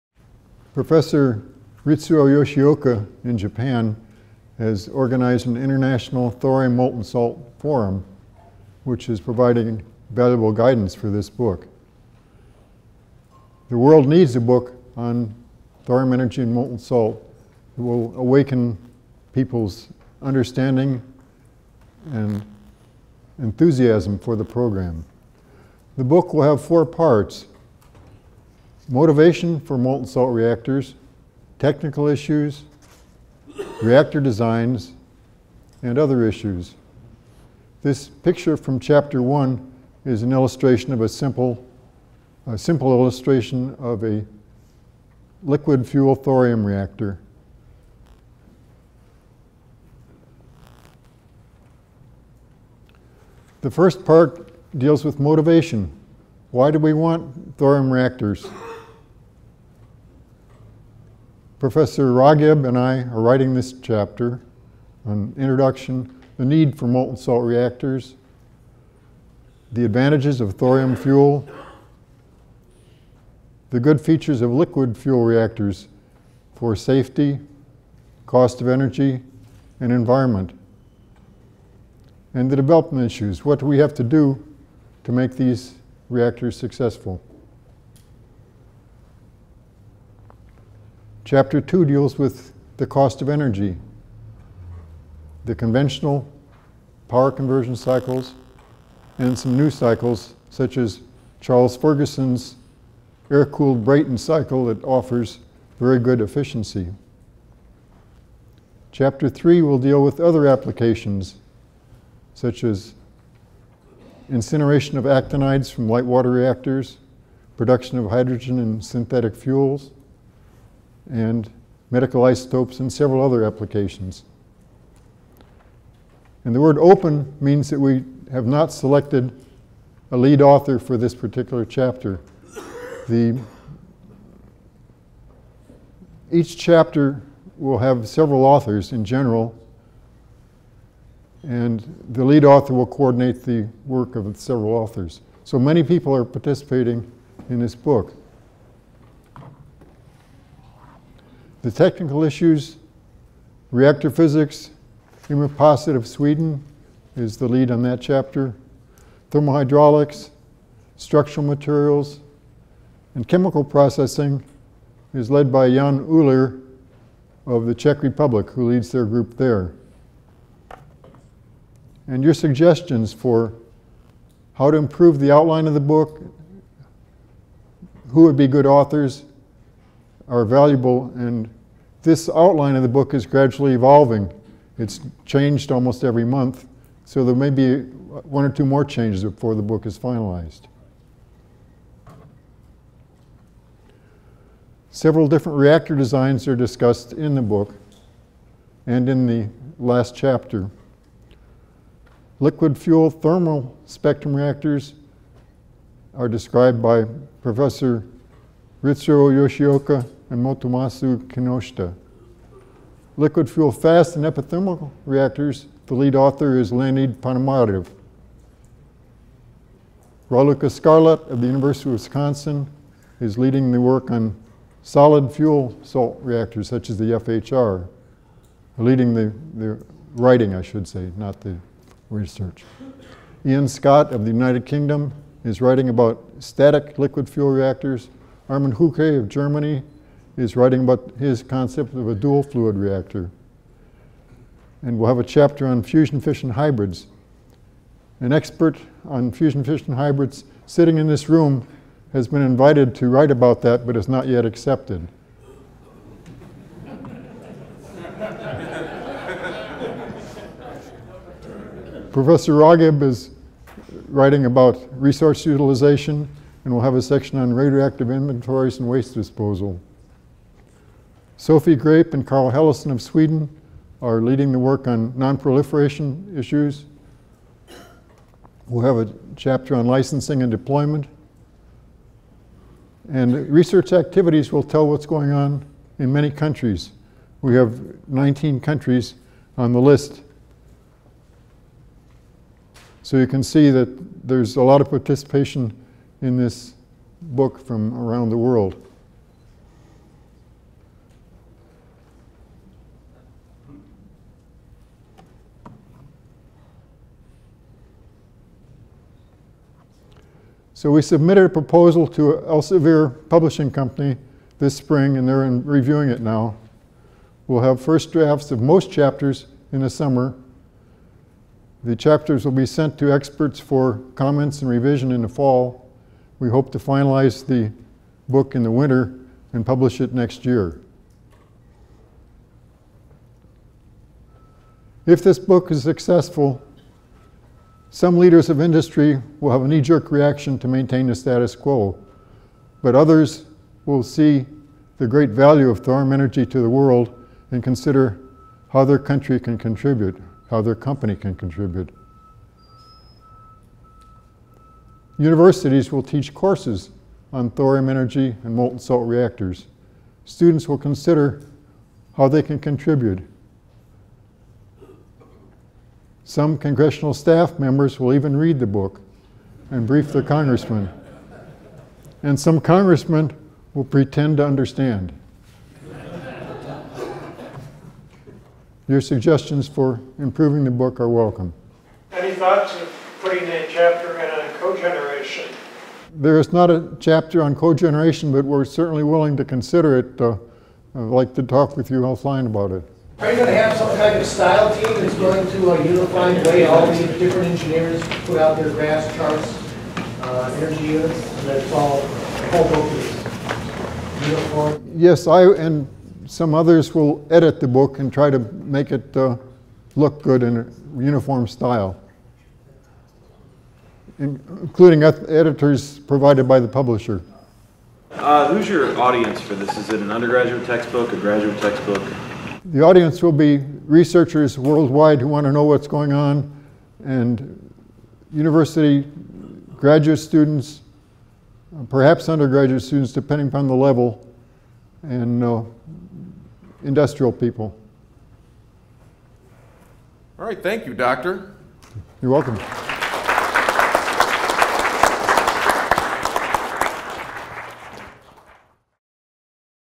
Book on Molten Salt Reactors -by Thomas Dolan @ TEAC7
Prof. Dolan has done nuclear fusion research and is the Editor of "Magnetic Fusion Technology" (Springer 2014).

He has worked at universities, in national labs, and at the International Atomic Energy Agency. Now he is organizing an international book on "Molten Salt Nuclear Reactors", with input from 18 countries.